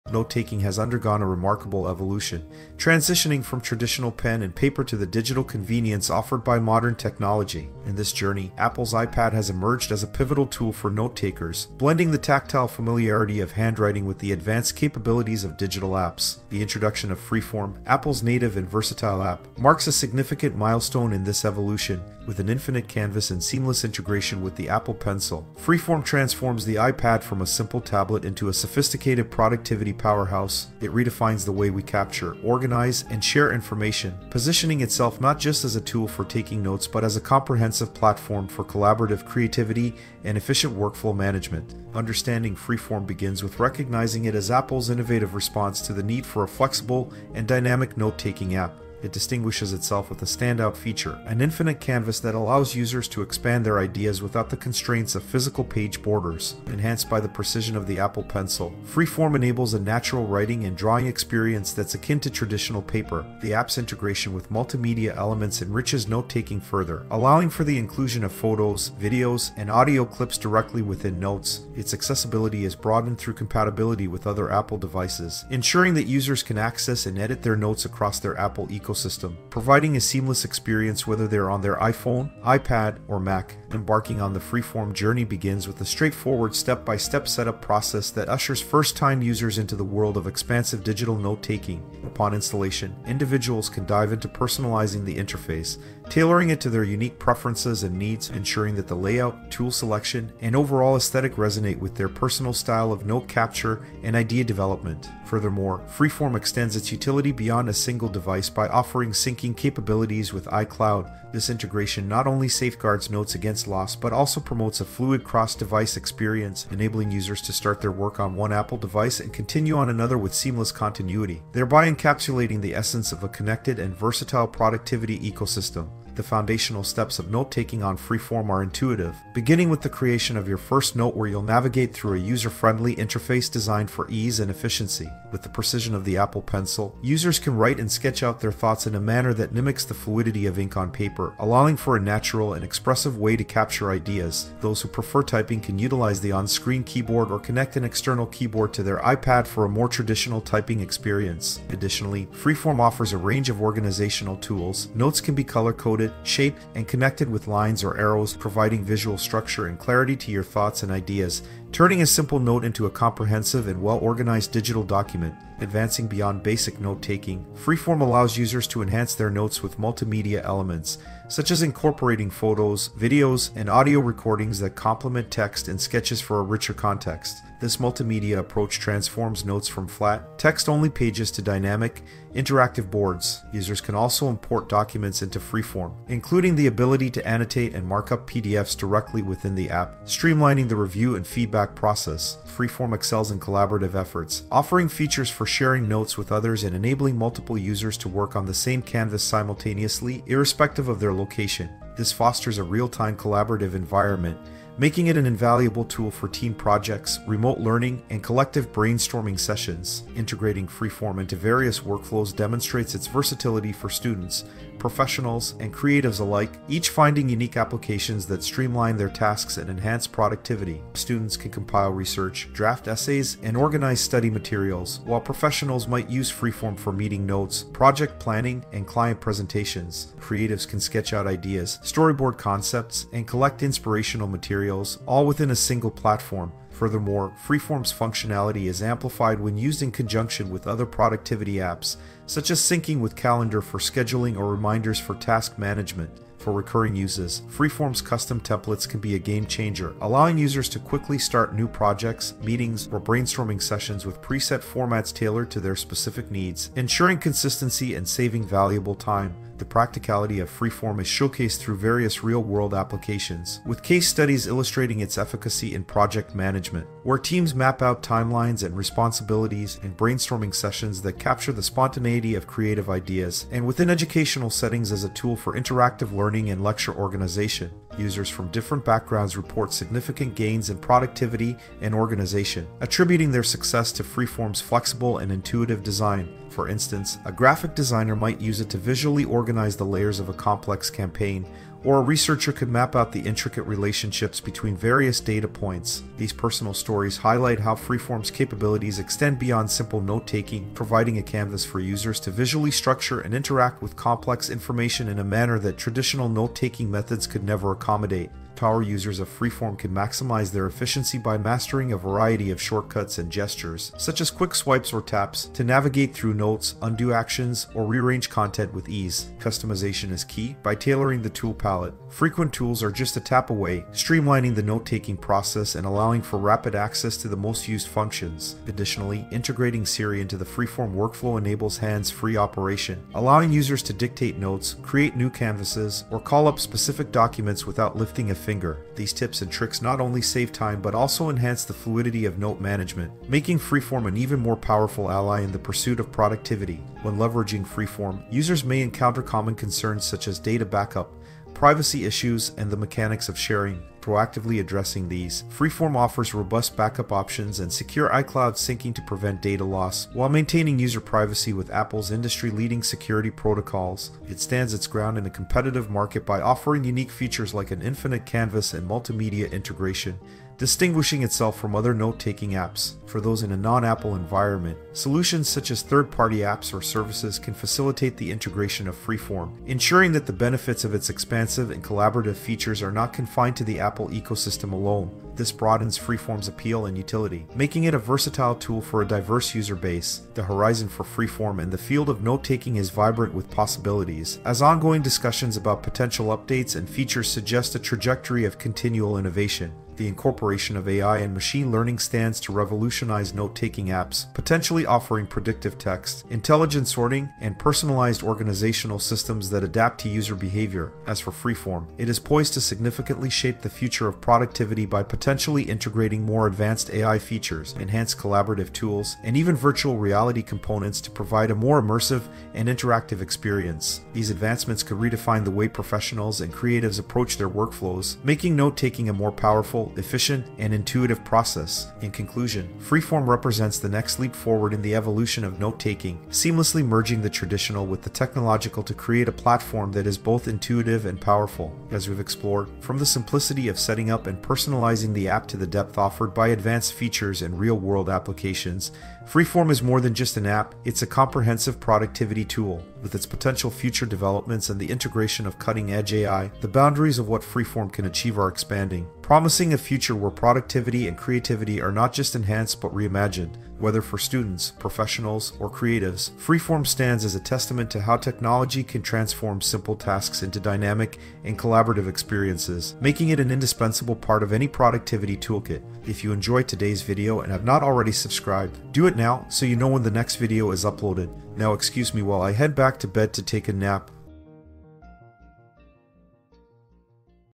How I Use Apple Freeform On iPad iOS for Note Taking and Productivity
Unlock the full potential of your iPad with our comprehensive guide to using Freeform, Apple's versatile note-taking app. This video is your ultimate resource for transforming your iPad into a productivity powerhouse. We delve deep into Freeform's features, from setting up and personalizing the app to advanced techniques for note management, multimedia integration, and collaboration. Whether you're a student, a professional, or a creative, you'll discover practical tips and real-world applications that will enhance your workflow and organization skills.